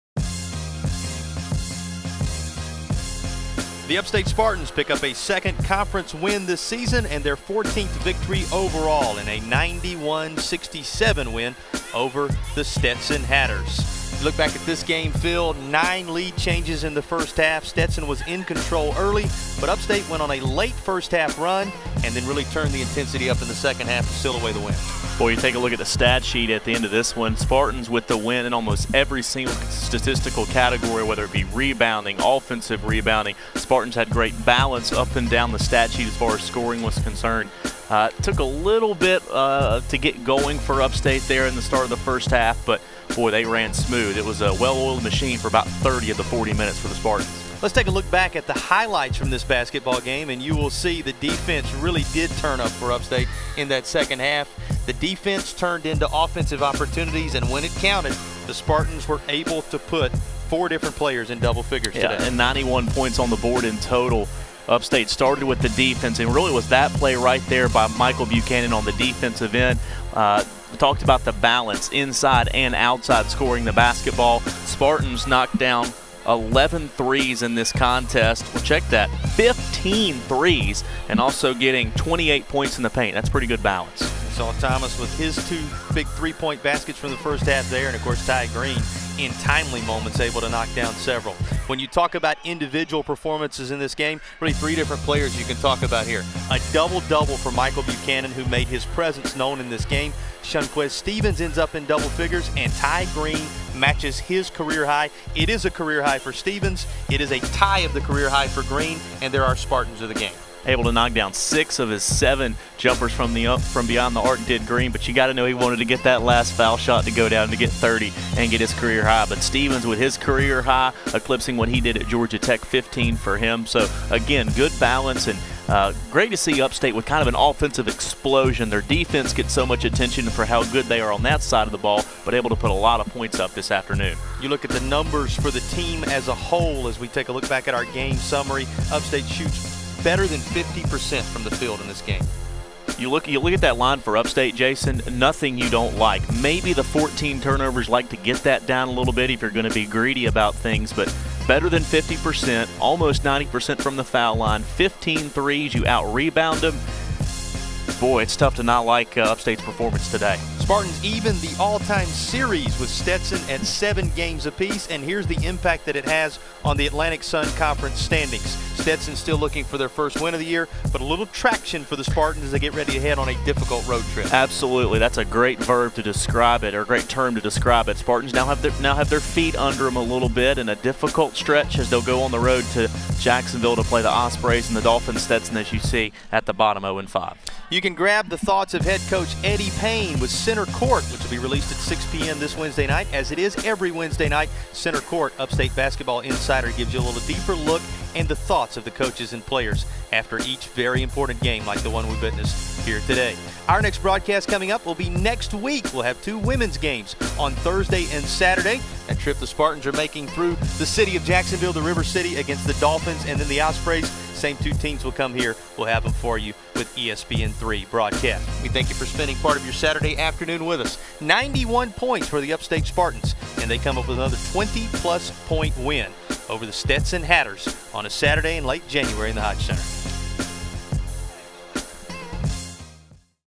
Upstate Men's Basketball vs. Stetson - 01/24/2014
Eddie Payne's Upstate Spartans get back in the win column with a 91-67 victory while Ty Greene ties his career-high record with 29 points.